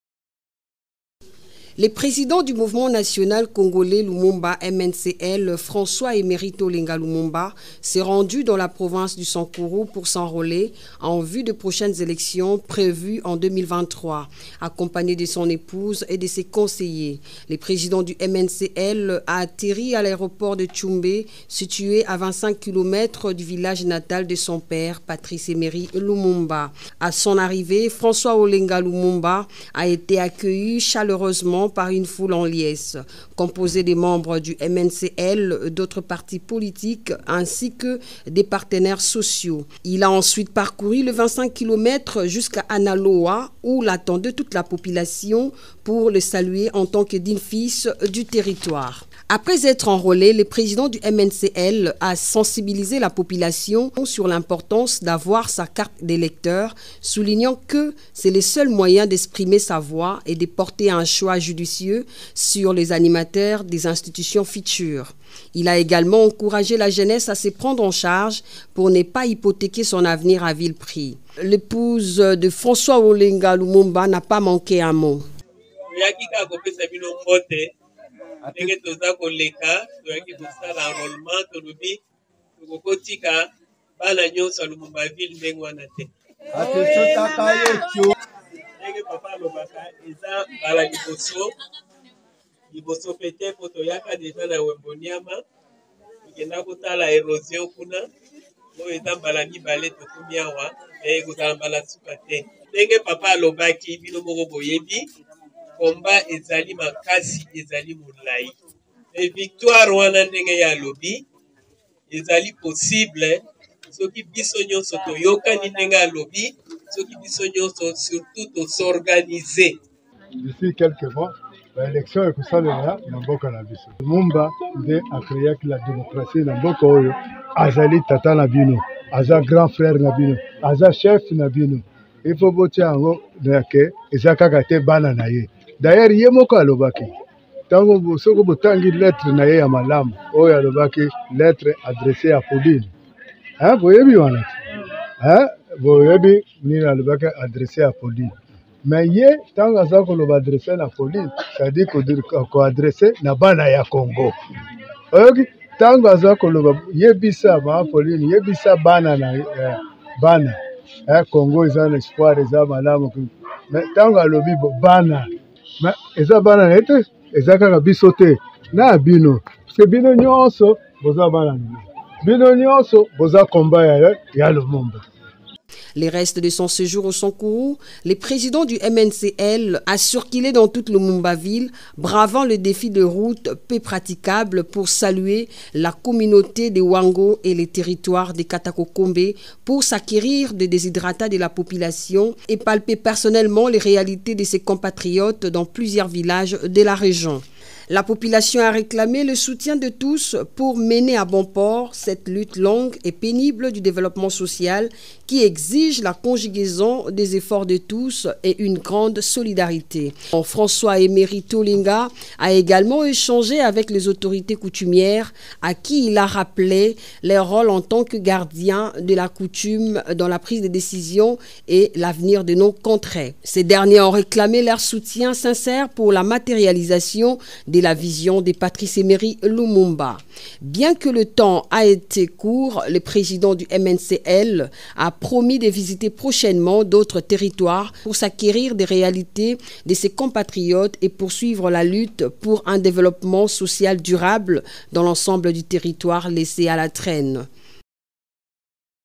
François Emery Tolenga Lumumba: L'héritage vivant du père de l'indépendance congolaise"
Plongez dans la vie et le parcours de François Emery Tolenga Lumumba, le président du Mouvement National Congolais pour la Liberté (MNCL) et fils de Patrice Lumumba, le héros de l'indépendance congolaise. Découvrez comment François perpétue l'héritage de son père à travers son engagement politique, sa vision pour un Congo libre et prospère, et son combat pour la justice sociale et la démocratie. Explorez l'influence de son père sur sa vie et son rôle en tant que leader dans la poursuite des idéaux de Patrice Lumumba.